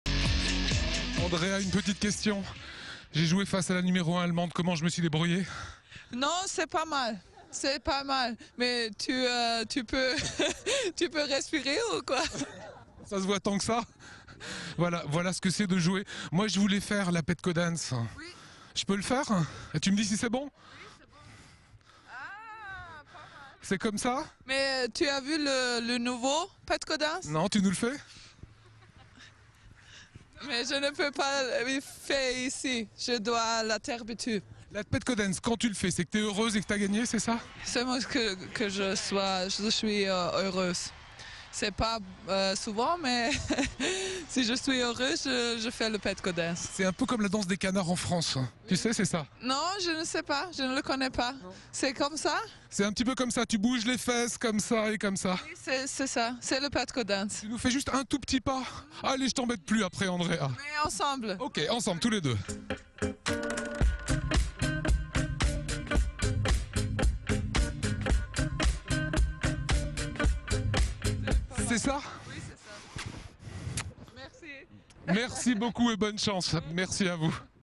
Tennis: la Petko-Dance, c'est quoi? (Strasbourg)
Sites webs/blogs, copiez cette vidéo avec le bouton "Intégrer" !
 Rencontre avec la joueuse de tennis Andrea Petkovic.